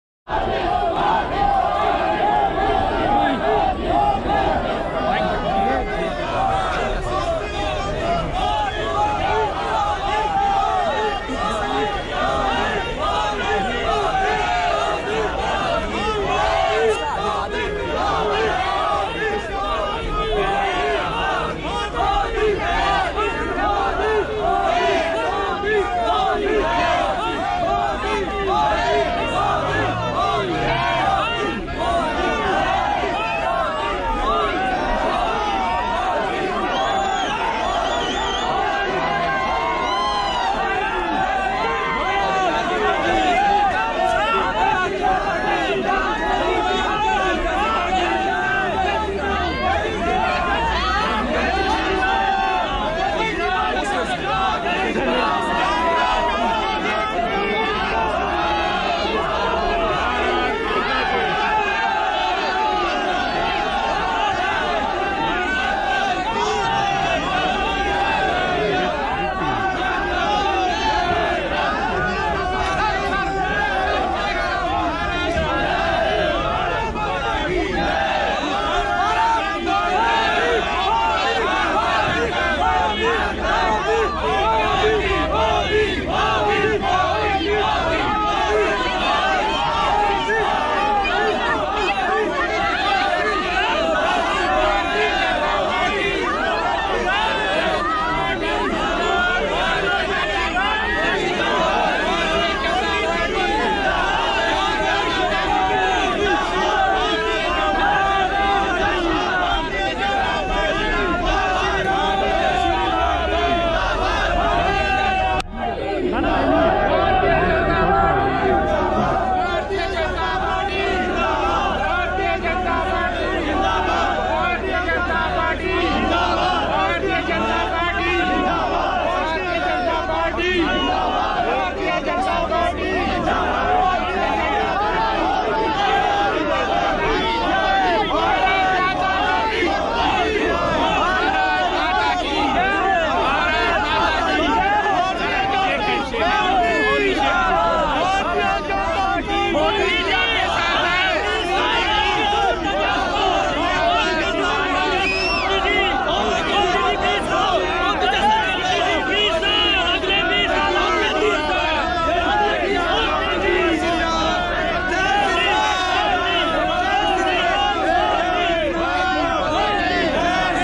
Memorable moments of PM Modi meeting Indian community in Rome, Italy | PMO
Memorable moments of PM Modi meeting Indian community in Rome, Italy